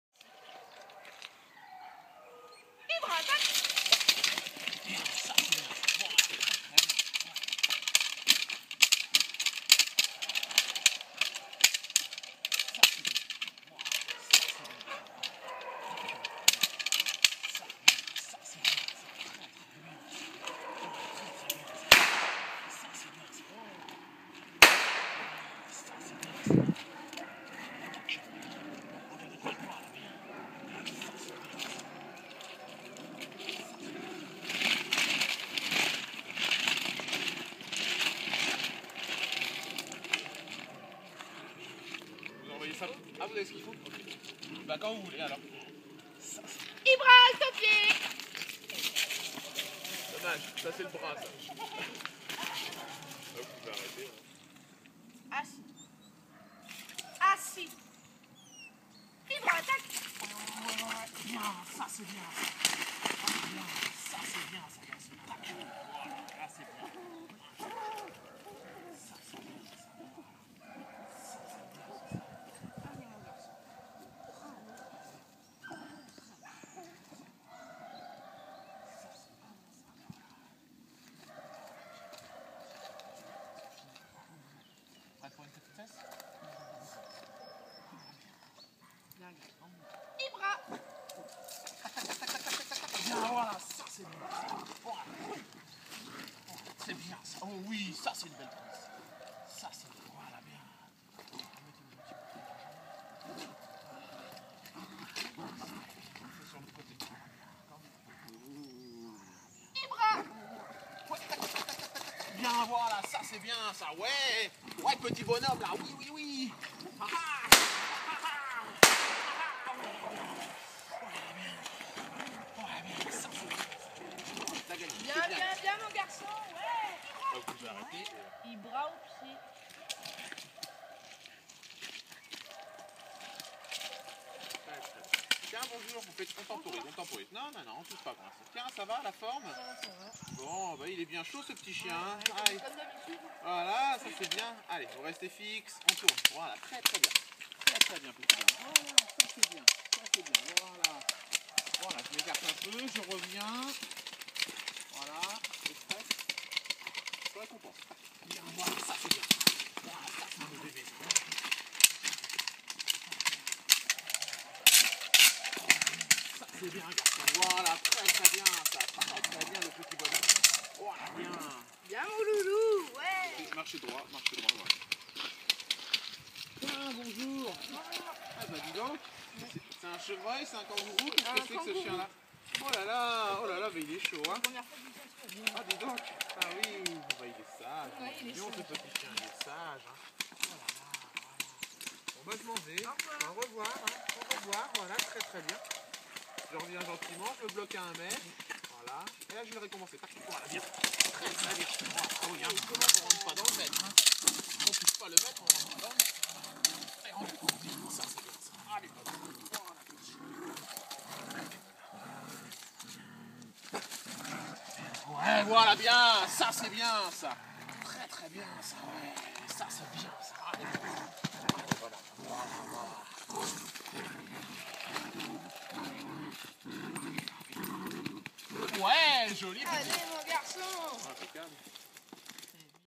Ibra chiot malinois de 4 mois en pension dressage
Ibra de la Cour du Roi Arthur est un chiot malinois LOF âgé de 4 mois.
C'est un fils du multi-finaliste ring 3 Boss et la Sélectifs ring 3 Bleide.
Ce chiot malinois est en pension dressage en ring à Passion Chien, tous les renseignements :
Fabrice BASNIER
PASSION CHIEN
Élevage des 2 Sabres